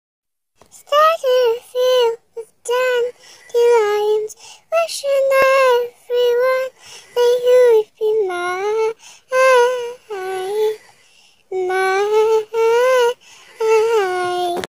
pls check the description thats why i made the vido
sorry for the bad spelling im not gonna post lately it might be short vidios but I'm going to make a gacha life movie the (dream smp)  and I'm trying to make it 1 hower or moor and soon I'm going on holaday for a week so won't be able to post so might have to wait a wile. have a good day or night it's night 🌃 for me lol 12:14pm